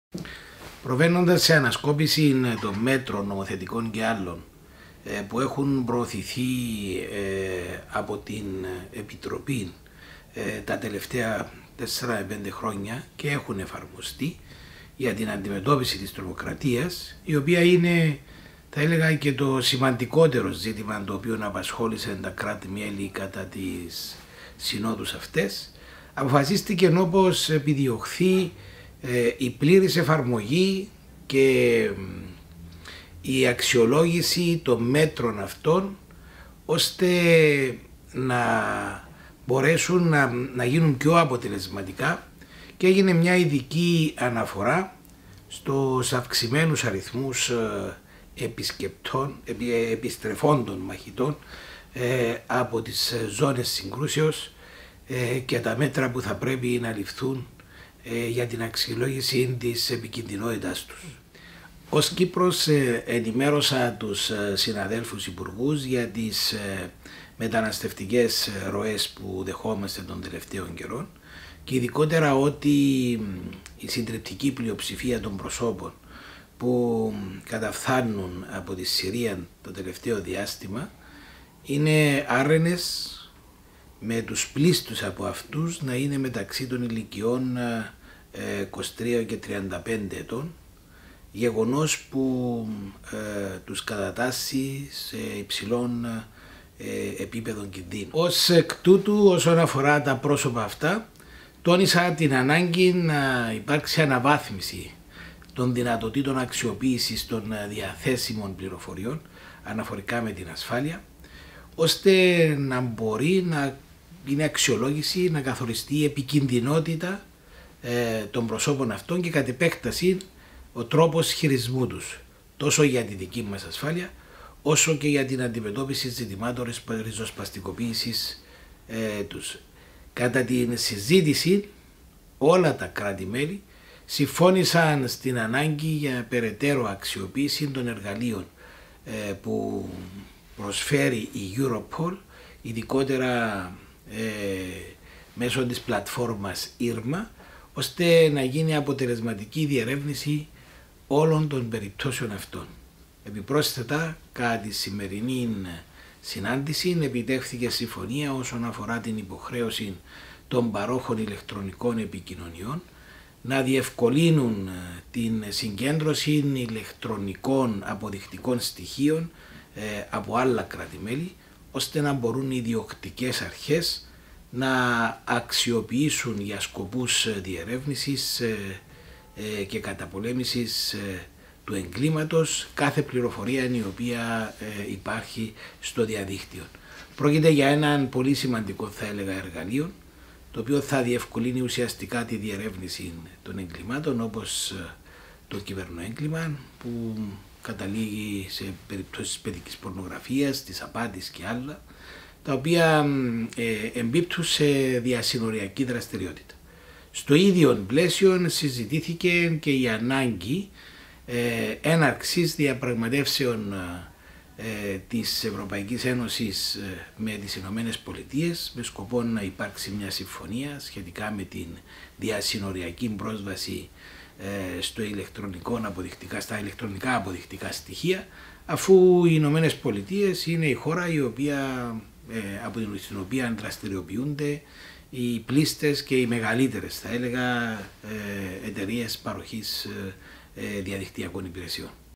EU Justice and Home Affairs Council (Brussels, 08/03/19) – Statement - Minister Nicolaou
Ο Υπουργός Δικαιοσύνης και Δημοσίας Τάξεως, κ. Ιωνάς Νικολάου συμμετέχει στο Συμβούλιο Δικαιοσύνης και Εσωτερικών Υποθέσεων της ΕΕ
Βρυξέλλες, 7-8 Μαρτίου 2019

The Minister of Justice and Public Order, Mr Ionas Nicolaou at the Justice and Home Affairs Council of the EU
Brussels, 7-8 Μarch 2019